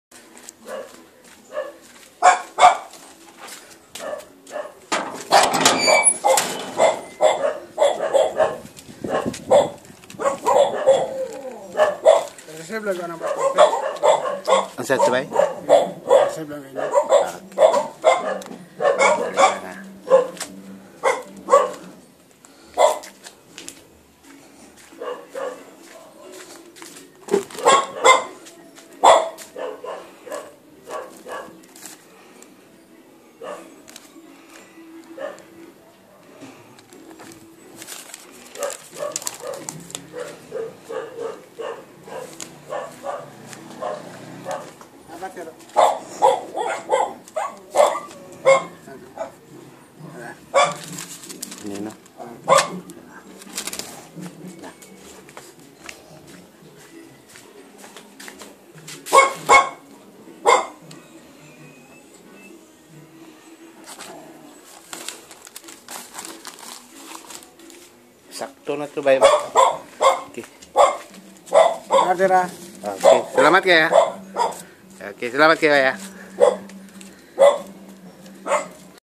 Invicta Speedway Men's Silver Stainless Steel Strap Watch 9223 (Cash On Delivery)from Lzada
Invicta Speedway Men's Silver Stainless Steel Strap Watch 9223   from Lazada Philippines. as off January 27,2016. this watch cost around php 5600 pesos from Lazada Philippines online store. if your lucky you could buy this at a half price, during promotional sale. bog-at ang relo, og tsada ang iya box, hadlok ko na madaot kung gamiton nako sa pag drive og motor kay basig maguba kung  masubrahan sa vibration sa manubilas motor. nakasulat sa box sticker ( MADE IN CHINA)